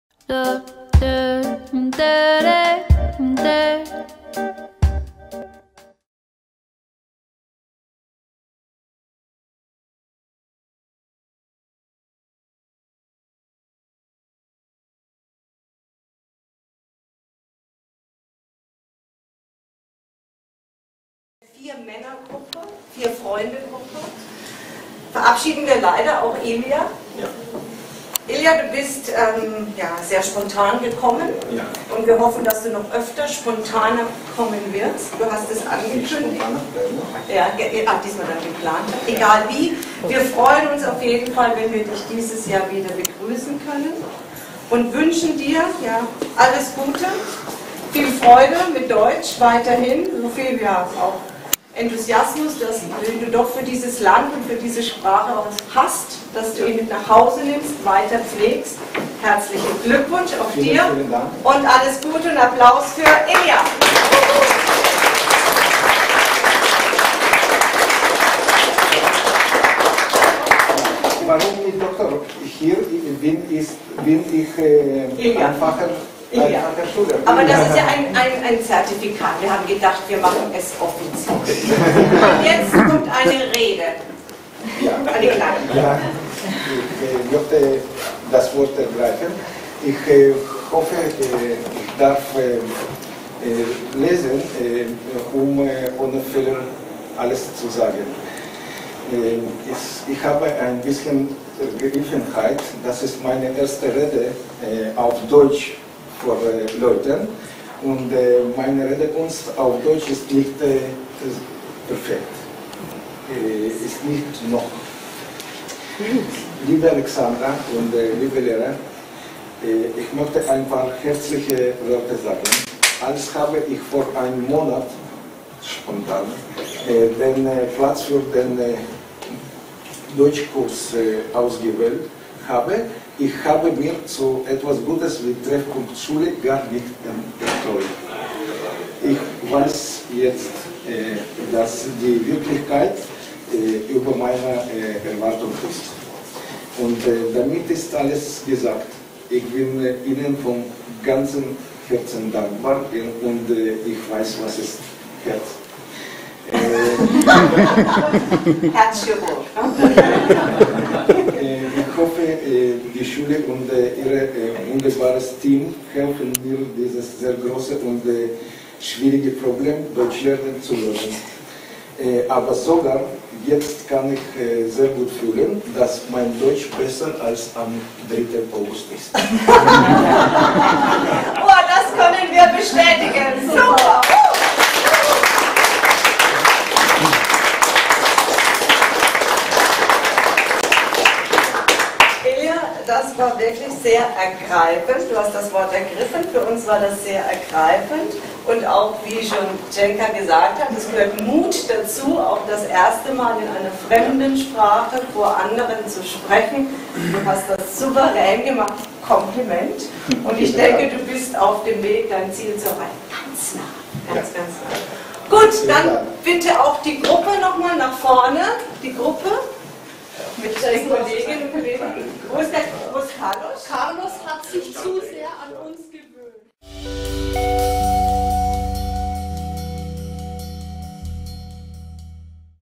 German certificate presentation and speech at Sprachinstitut TREFFPUNKT
Ilya from Jerusalem studied German in an IntensivePlus Course for two weeks at Sprachinstitut TREFFPUNKT.
During the certificate presentation he gave a short speech .
Recorded August, 14  2015 at Sprachinstitut TREFFPUNKT in Bamberg / Germany.